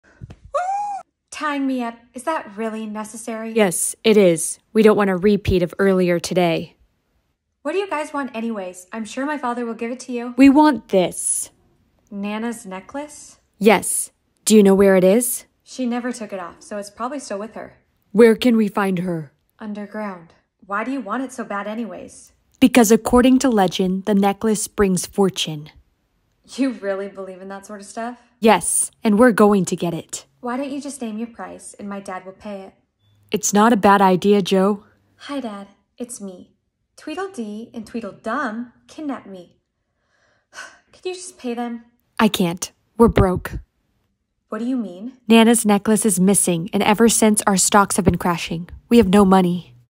Rich Girl’s Allowance PART 2
pov a rich girl doesn't get her allowance one day and she soon discovers why...